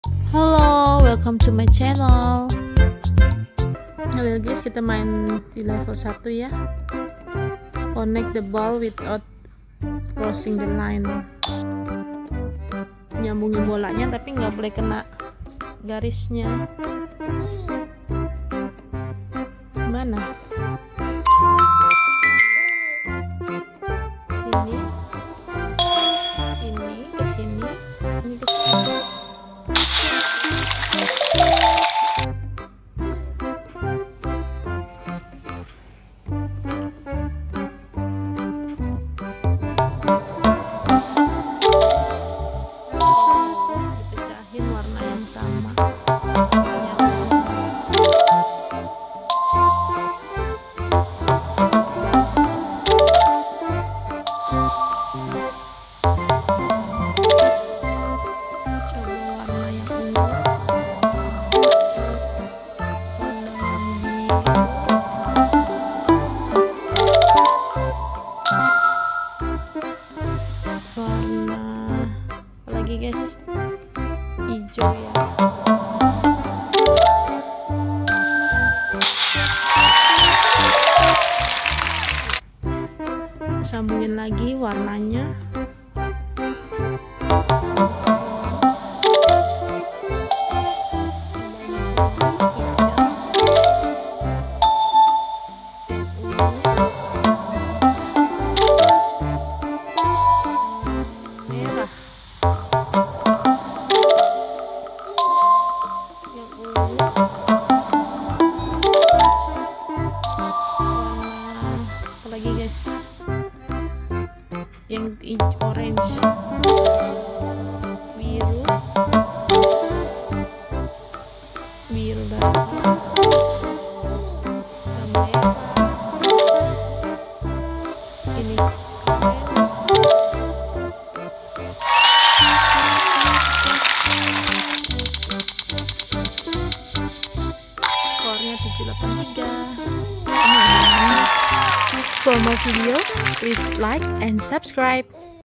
PERMAINAN NYAMBUNGIN TITIK#shorts
PERMAINAN Nyambungin Titik. Sudah siap untuk tantangan visual yang menyenangkan. SELAMAT MENCOBA!